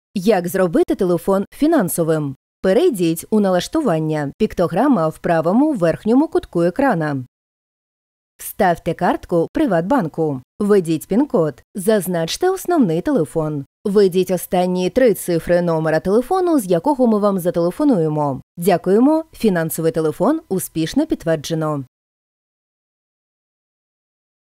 Як зробити телефон фінансовим у терміналі самообслуговування
Щоб зробити телефон фінансовим у терміналі самообслуговування, потрібно виконати такі дії:
1. Перейдіть у «Налаштування» (піктограма  в правому верхньому кутку екрана).
2. Вставте картку ПриватБанку.
3. Введіть ПІН-код.
4. Зазначте основний телефон.
5. Введіть останні три цифри номера телефону, з якого ми вам зателефонуємо.
6. Дякуємо! Фінансовий телефон успішно підтверджено.